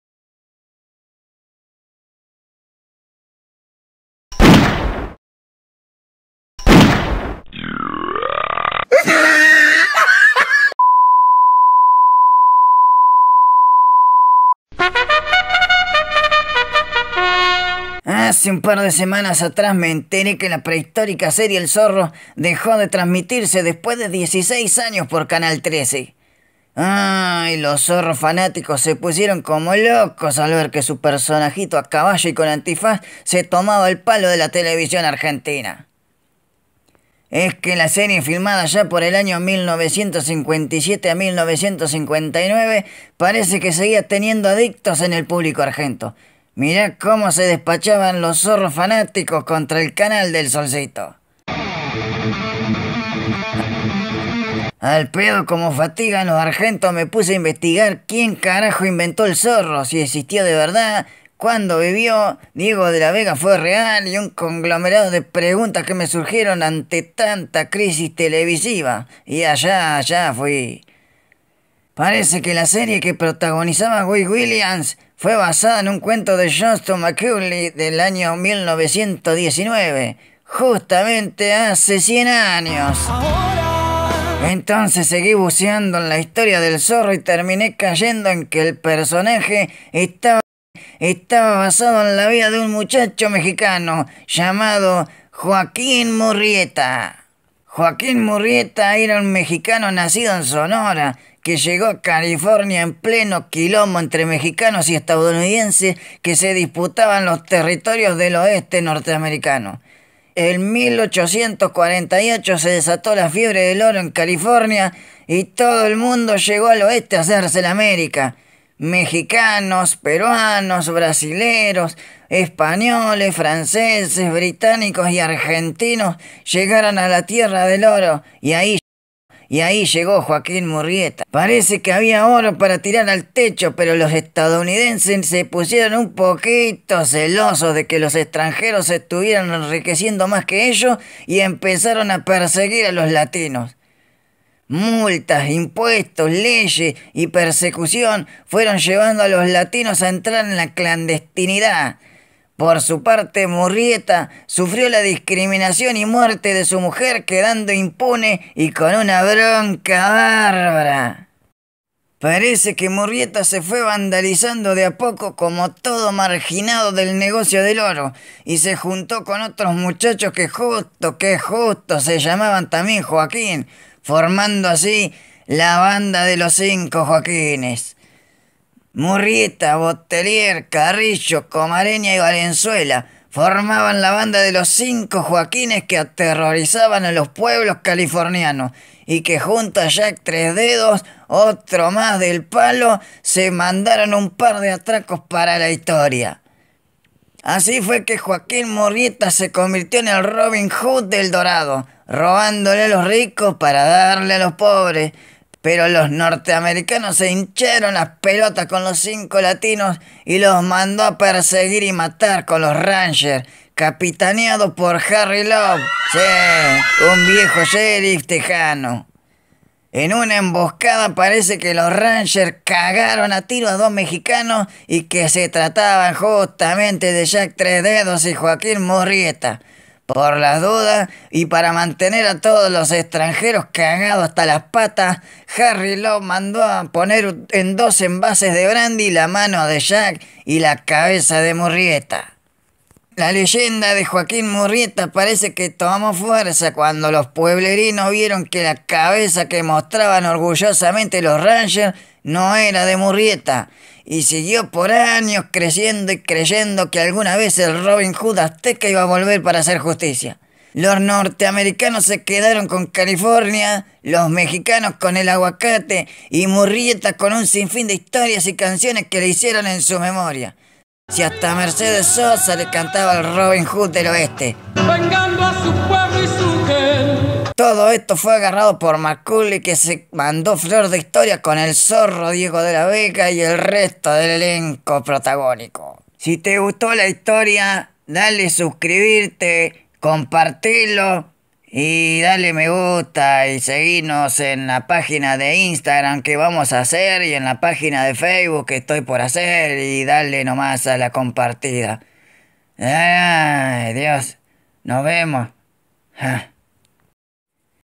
¿Existió El Zorro? / REVISTA PUAJ! N°1
Primera edición de la revista comenzando con una histórica serie. Y la verdadera historia detrás del personaje y sus orígenes contada de una forma poco peculiar pero efectiva.

GOZENLO!